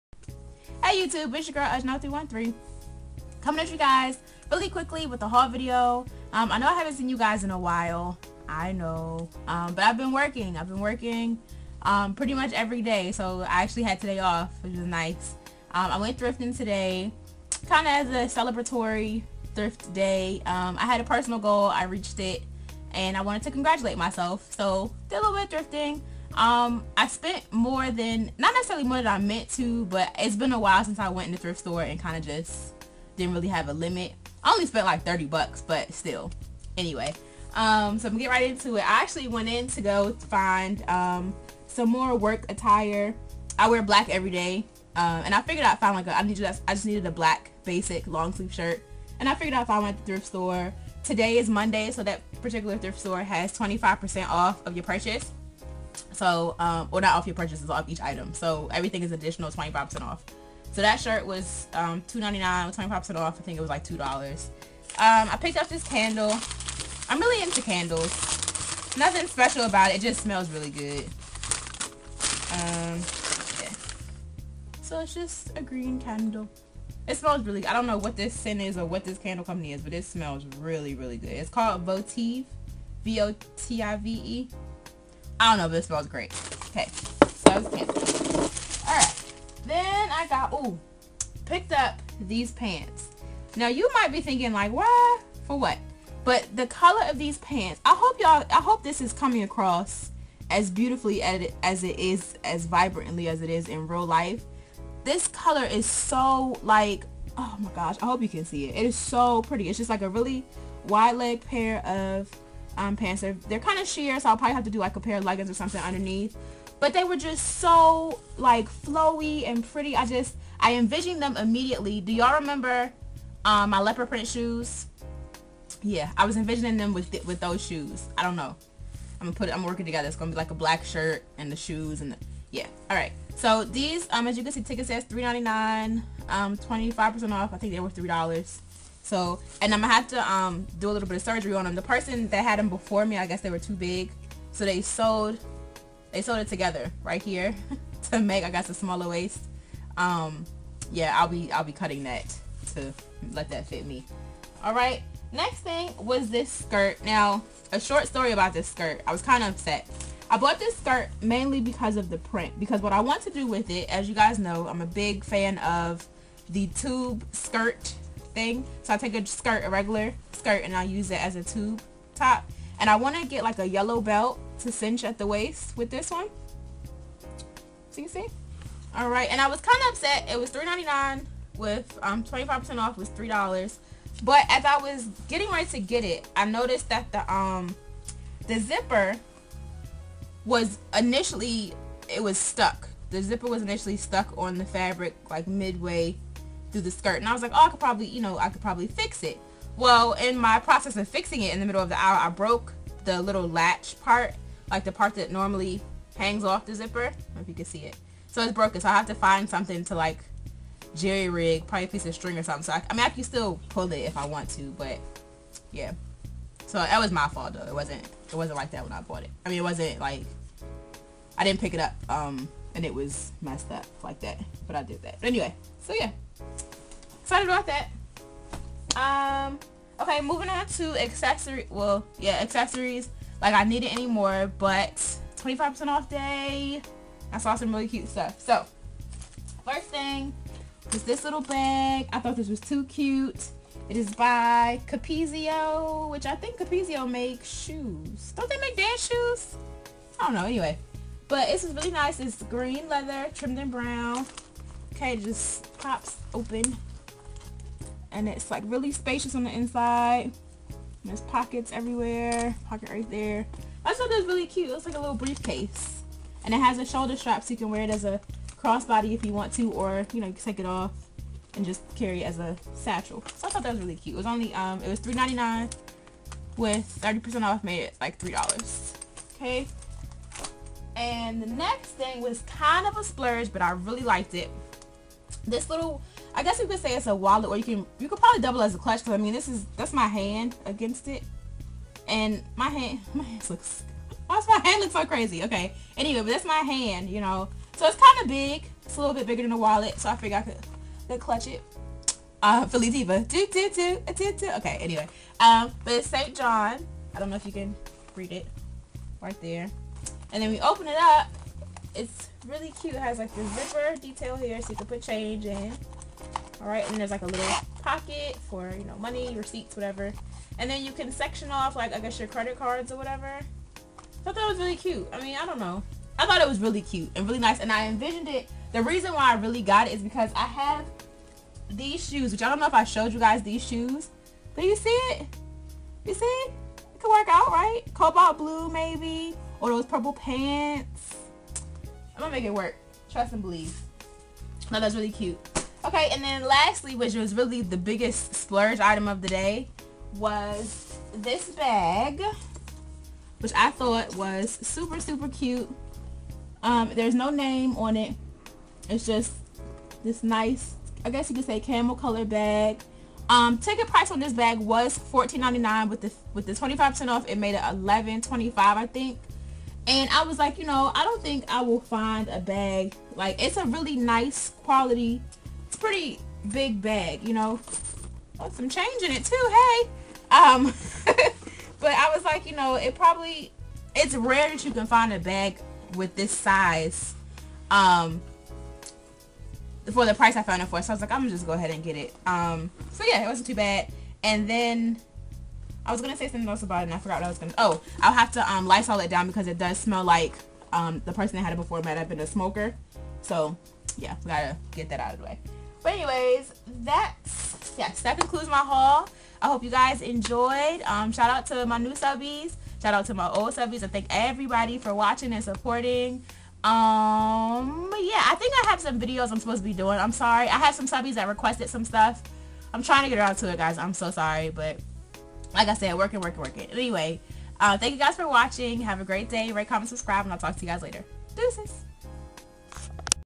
Thriftin...
Took myself on a little thrift trip...good stuff :o)

*I paid for everything w/ my own coins!!*